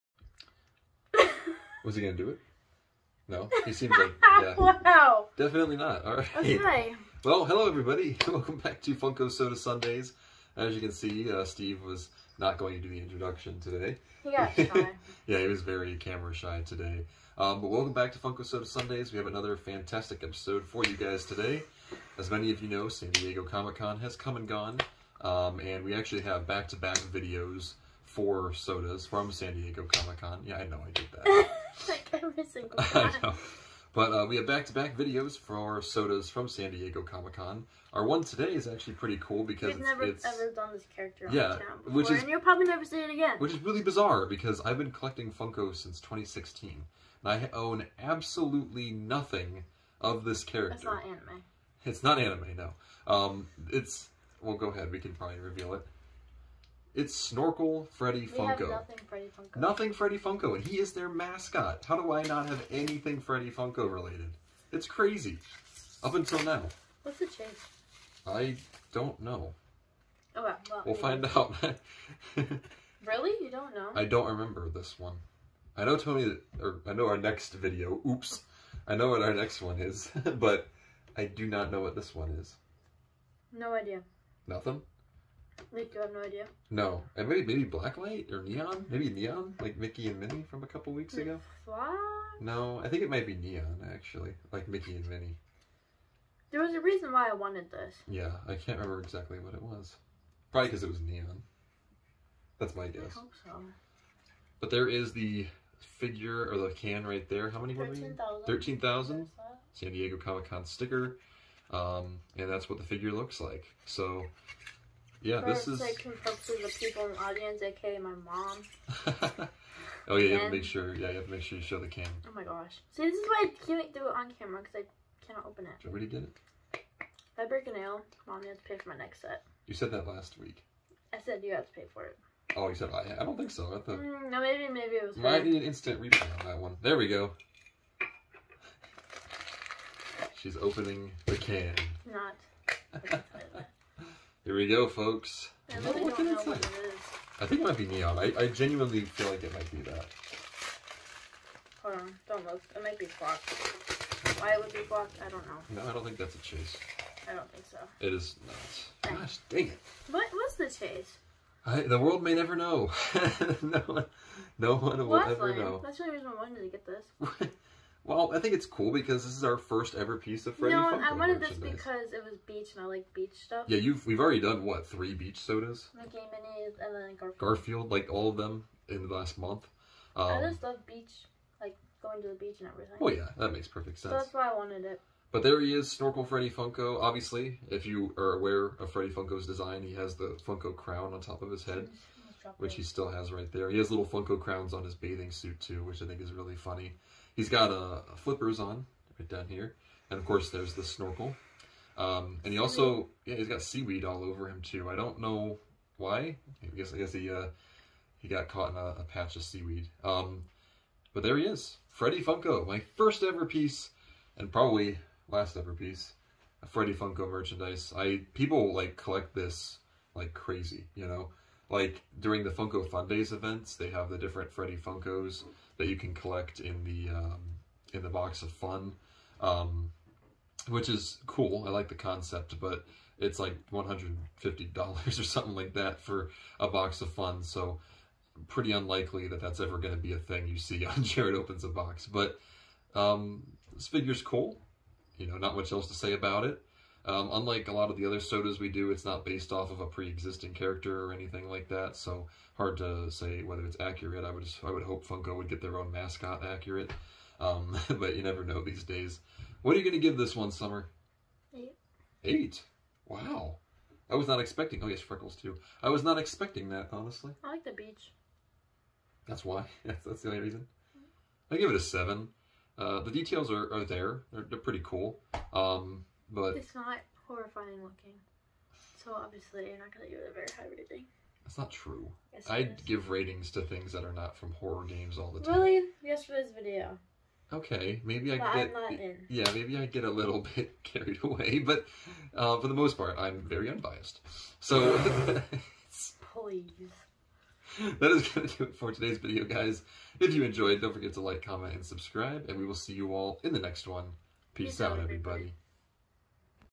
Funko Soda Sunday: Snorkel Freddy Funko!
Our review of the Snorkel Freddy Funko Soda figure!